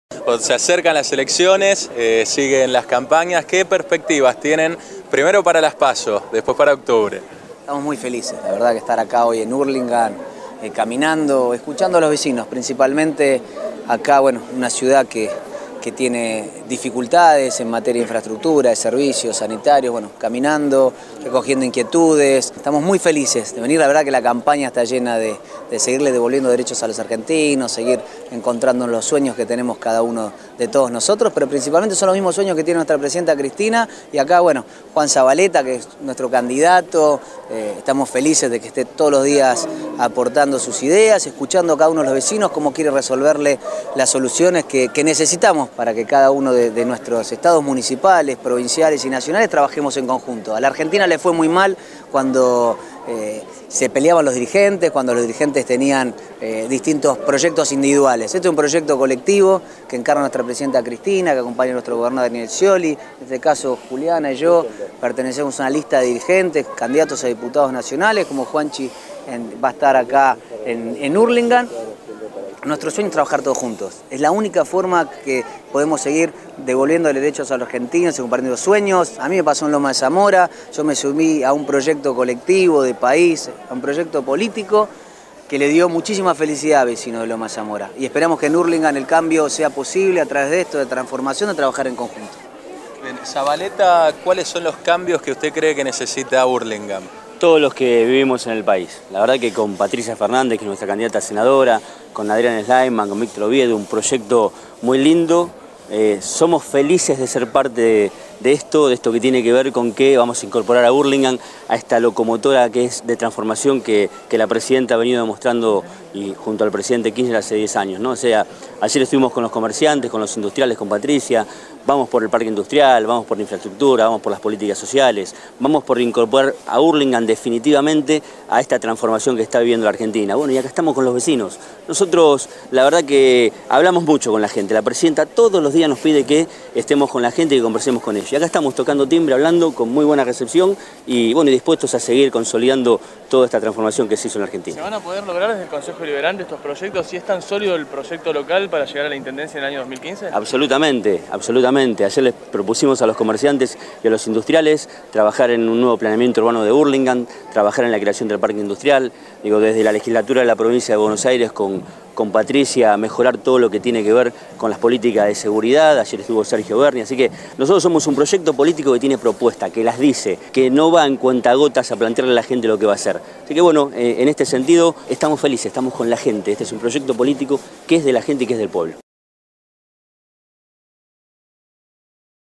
Insaurralde: "A la Argentina le fue mal con los proyectos individuales"
Martín Insaurralde y Juliana Di Tullio caminaron con el primer candidato a concejal del FpV en Hurlingham, Juan Zabaleta. "Esta lista muestra que es un proyecto colectivo con propuestas", dijeron.